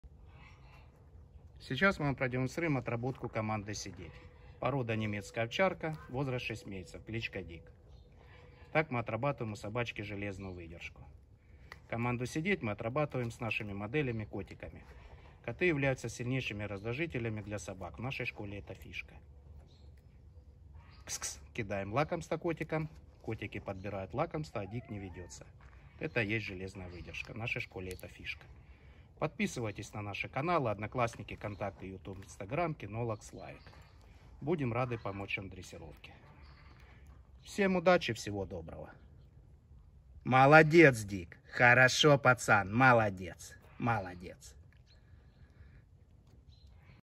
Немецкая овчарка 6 месяцев Дик! Команда сидеть с котами! Железная выдержка!
Немецкая овчарка дрессировка